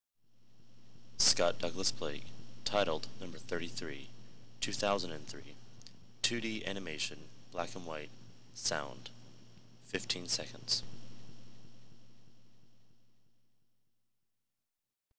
Titled No. 33 Animation
Conceptual art about title cards where the medium and size are self explanatory. I am interested in my desire to make art and also how my artworks function as commodities.